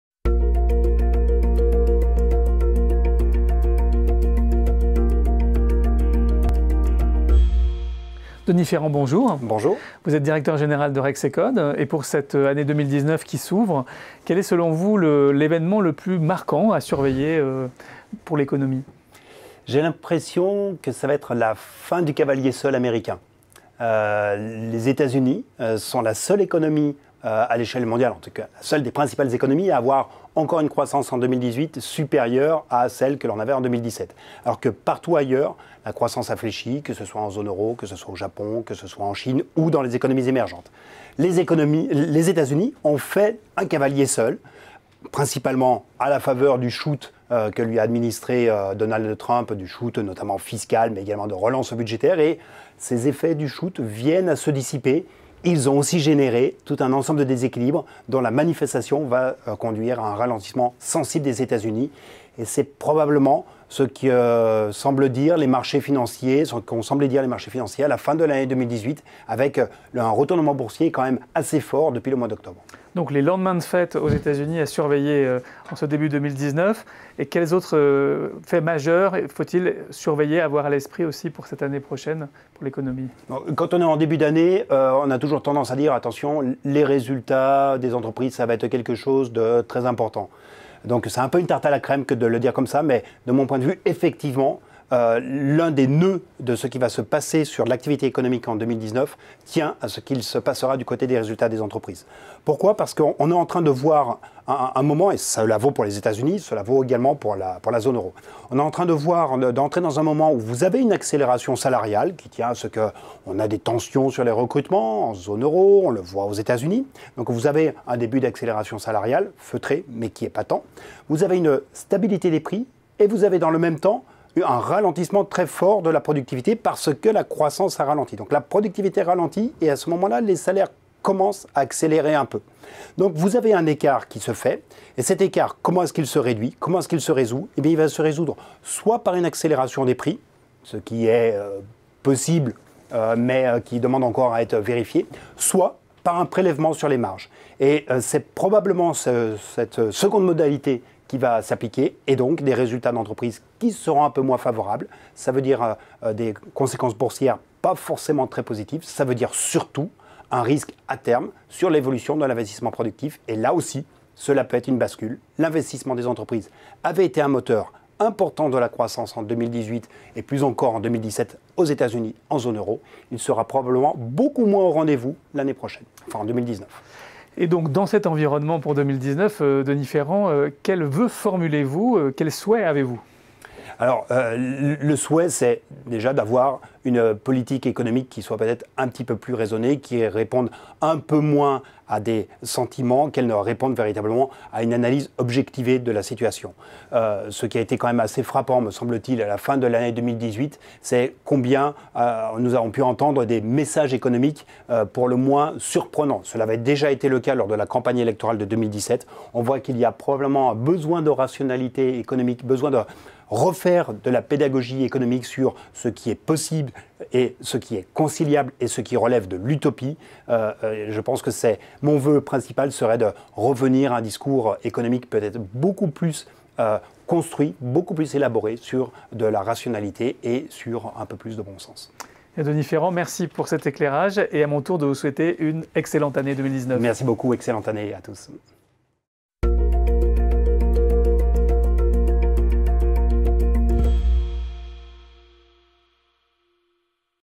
2019 : La fin du cavalier seul américain et d'un cycle économique [Denis Ferrand]
Xerfi Canal a reçu Denis Ferrand, Directeur général de Coe-Rexecode, pour parler de l'économie américaine en 2019.
Une interview menée par Adrien de Tricornot.